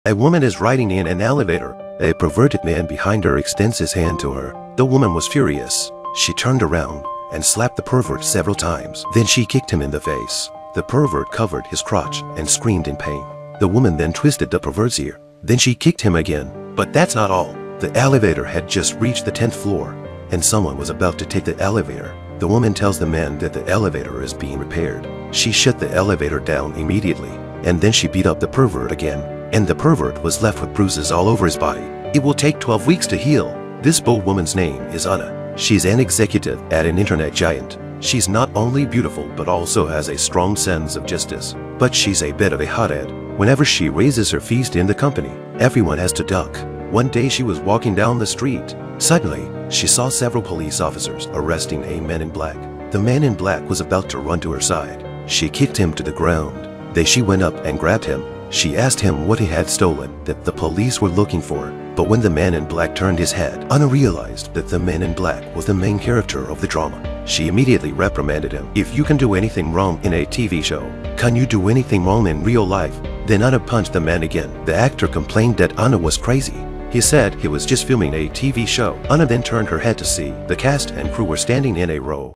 Woman sexually harassed in elevator, fights back violently against man!
Woman sexually harassed in elevator, fights back violently against man!film